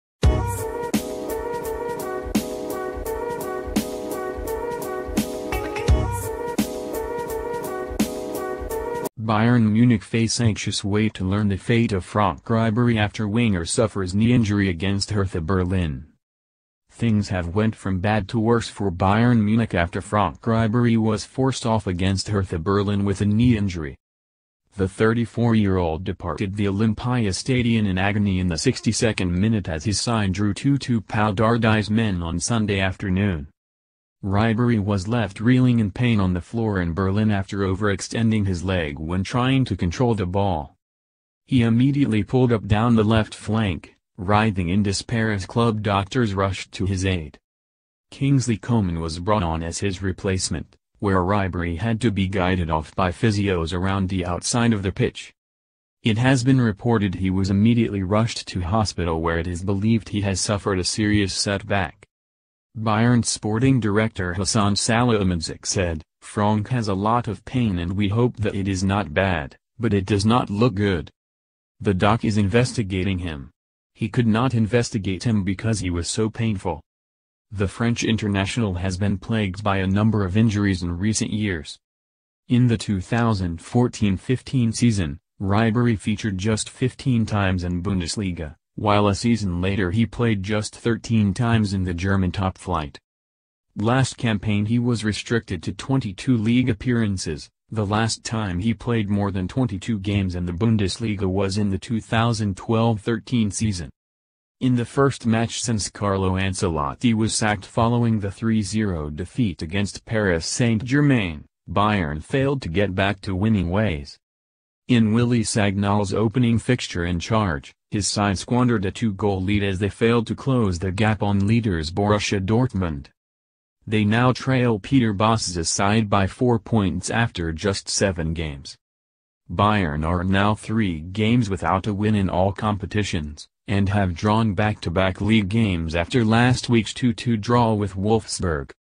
Bayern Munich face anxious wait to learn the fate of Franck Ribery after winger suffers knee
Bayern Munich face anxious wait to learn the fate of Franck Ribery after winger suffers knee injury against Hertha Berlin.
THINGS have went from bad to worse for Bayern Munich after Franck Ribery was forced off against Hertha Berlin with a knee injury.

The 34-year-old departed the Olympiastadion in agony in the 62nd minute as his side drew 2-2 Pal Dardai&x27;s men on Sunday afternoon.

Ribery was left reeling in pain on the floor in Berlin after overextending his leg when trying to control the ball.